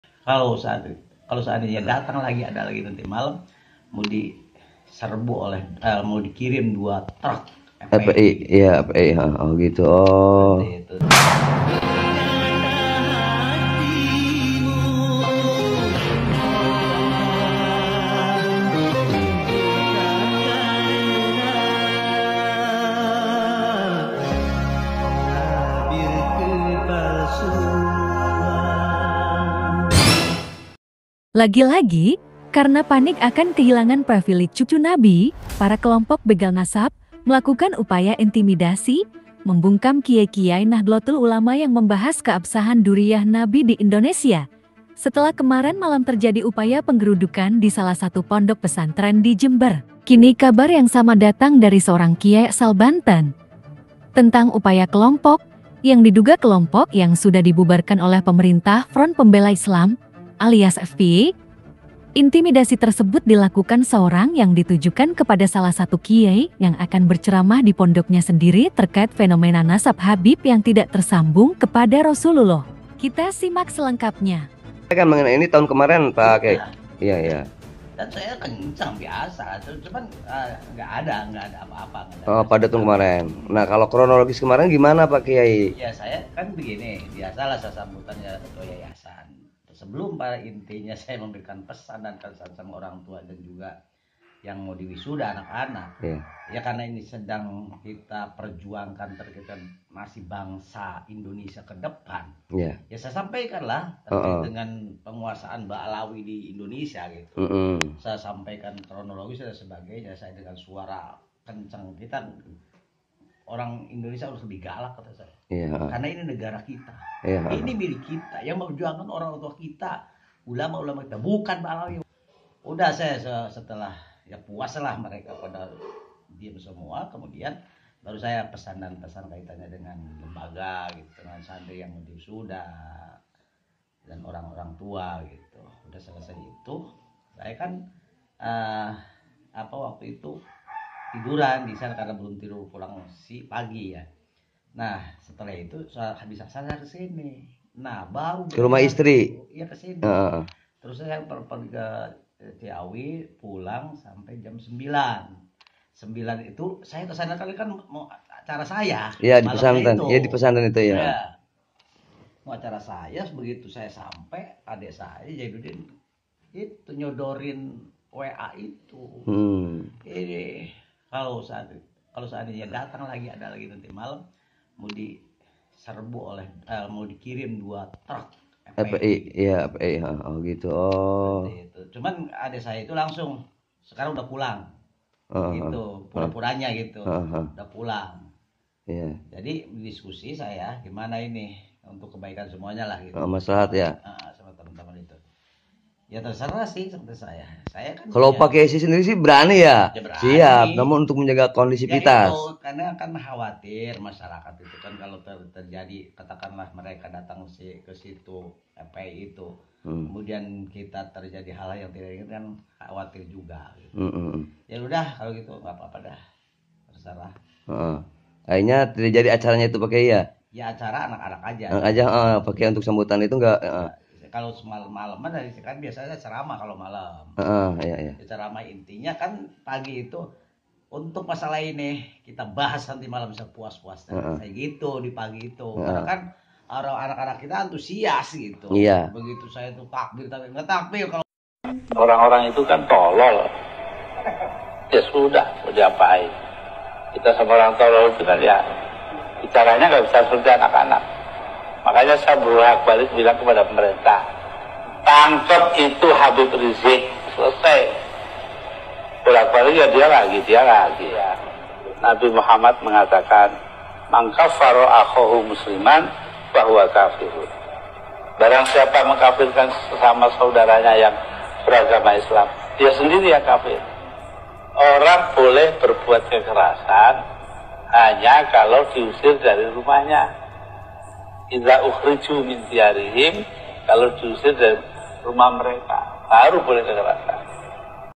GEROMBOLAN FPI REBOND SIAPKAN 2 TRUK GRODUK PONDOK KIAI BANTEN INI!
kelompok yang didirikan riziq SIHAB ini terus bergerilya walau sudah di bubarkan oleh pemerintah!